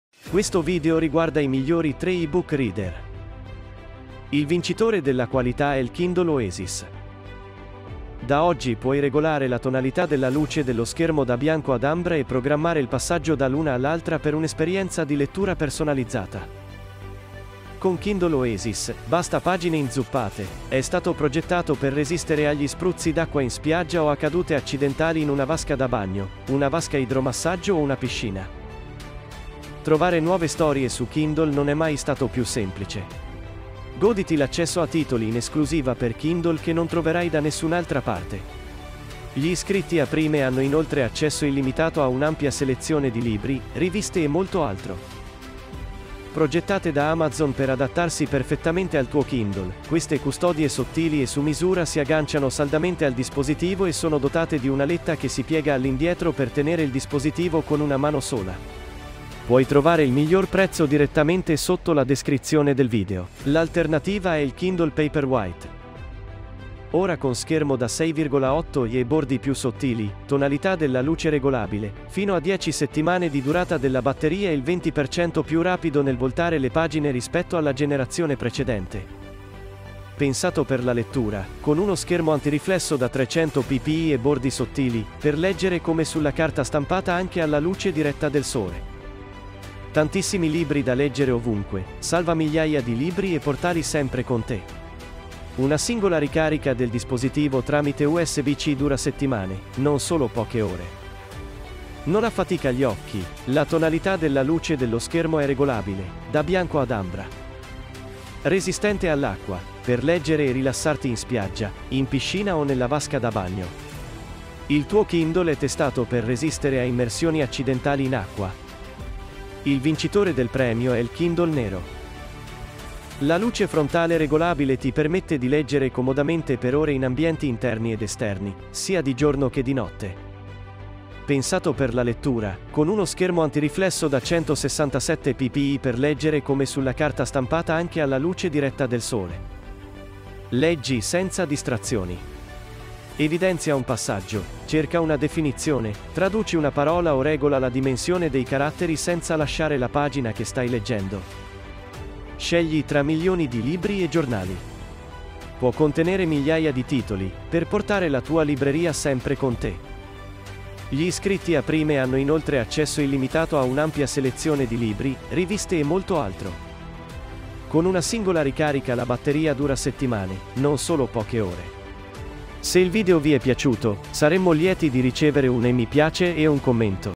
✅ Le Migliori | Ebook Reader 2023
Il vincitore della qualità è l' Kindle Oasis.

Da oggi puoi regolare la tonalità della luce dello schermo da bianco ad ambra e programmare il passaggio dall'una all’altra per un’esperienza di lettura personalizzata.

Con Kindle Oasis, basta pagine inzuppate: è stato progettato per resistere agli spruzzi d’acqua in spiaggia o a cadute accidentali in una vasca da bagno, una vasca idromassaggio o una piscina.

Trovare nuove storie su Kindle non è mai stato più semplice. Goditi l’accesso a titoli in esclusiva per Kindle che non troverai da nessun’altra parte. Gli iscritti a Prime hanno inoltre accesso illimitato a un’ampia selezione di libri, riviste e molto altro.

Progettate da Amazon per adattarsi perfettamente al tuo Kindle, queste custodie sottili e su misura si agganciano saldamente al dispositivo e sono dotate di un’aletta che si piega all’indietro per tenere il dispositivo con una mano sola.

L'alternativa è il Kindle Paperwhite.

Ora con schermo da 6,8’’ e bordi più sottili, tonalità della luce regolabile, fino a 10 settimane di durata della batteria e il 20% più rapido nel voltare le pagine rispetto alla generazione precedente.
Pensato per la lettura, con uno schermo antiriflesso da 300 ppi e bordi sottili, per leggere come sulla carta stampata anche alla luce diretta del sole.
Tantissimi libri da leggere ovunque, salva migliaia di libri e portali sempre con te. Una singola ricarica del dispositivo tramite USB-C dura settimane, non solo poche ore.
Non affatica gli occhi, la tonalità della luce dello schermo è regolabile, da bianco ad ambra.
Resistente all’acqua, per leggere e rilassarti in spiaggia, in piscina o nella vasca da bagno. Il tuo Kindle è testato per resistere a immersioni accidentali in acqua.
Il vincitore del premio è l' Kindle Nero.

La luce frontale regolabile ti permette di leggere comodamente per ore in ambienti interni ed esterni, sia di giorno che di notte.
Pensato per la lettura, con uno schermo antiriflesso da 167 ppi per leggere come sulla carta stampata anche alla luce diretta del sole.
Leggi senza distrazioni. Evidenzia un passaggio, cerca una definizione, traduci una parola o regola la dimensione dei caratteri senza lasciare la pagina che stai leggendo.
Scegli tra milioni di libri e giornali. Può contenere migliaia di titoli, per portare la tua libreria sempre con te.
Gli iscritti a Prime hanno inoltre accesso illimitato a un’ampia selezione di libri, riviste e molto altro.
Con una singola ricarica la batteria dura settimane, non solo poche ore.